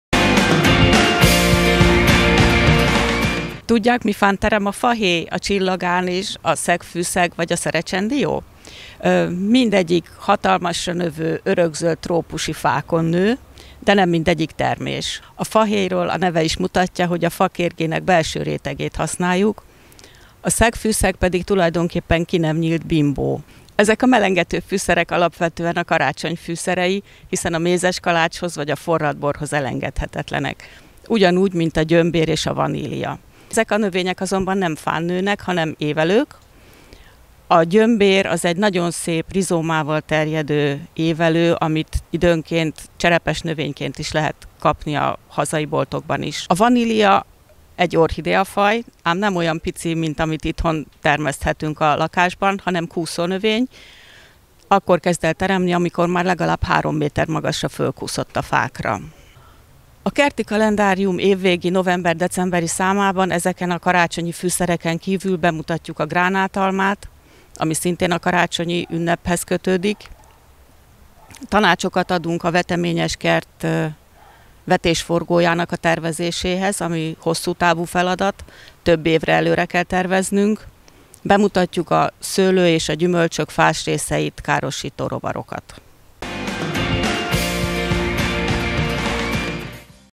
MMG - Mi fán terem? (A melengető téli fűszerek eredete)
A Kerti Kalendárium november-decemberi számában a melengető téli fűszerek eredetét ismertetjük. A fahéjról a neve is elárulja, hogy tulajdonképpen fakéreg, amit egy nagy termetű, örökzöld trópusi fáról fejtenek le. Hasonlóképp trópusi fán terem a szerecsendió és a szegfűszeg is, ami viszont nem termés, hanem ki sem nyílt virágbimbó. Bemutatunk a díszkertbe is ültethető színes és ehető bogyójú cserjéket, a gránátalmát, és tanácsokat adunk a zöldségeskert vetésforgójának tervezéséhez.
(Készítette: Horváth Csilla, Dudás Ágnes)